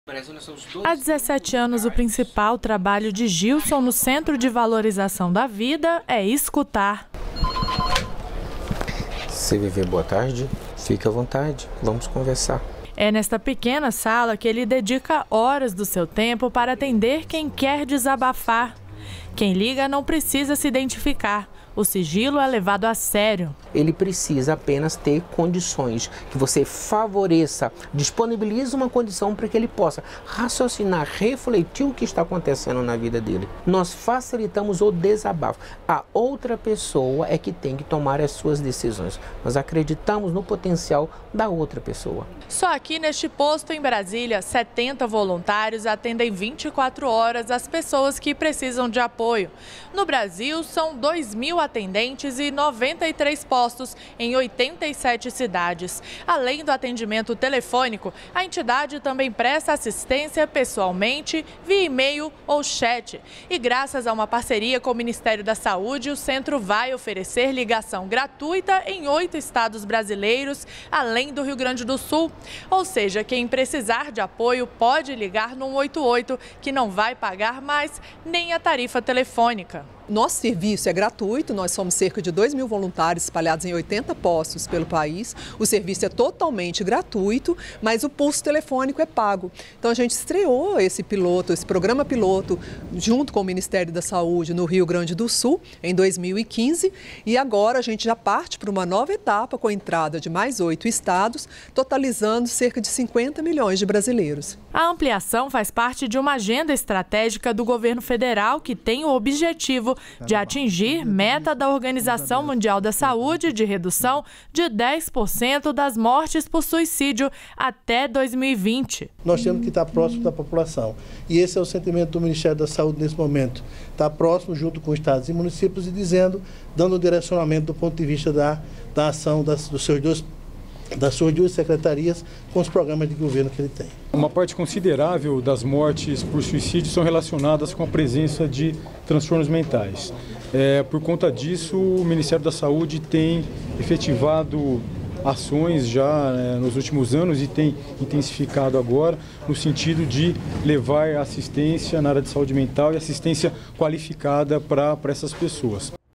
Dados inéditos do Ministério da Saúde apontam que o suicídio é a quarta causa de morte entre jovens
REPÓRTER NBR 20H – 21.09.17: Governo federal divulgou o primeiro boletim epidemiológico sobre suicídio no Brasil. Considerado como um problema de saúde pública, o suicídio é a segunda causa de morte no mundo entre jovens de 15 a 29 anos. Segundo o Centro de Valorização à Vida, de 10 casos que acontecem, 9 poderiam ser evitados, caso a pessoa tivesse algum tipo de apoio.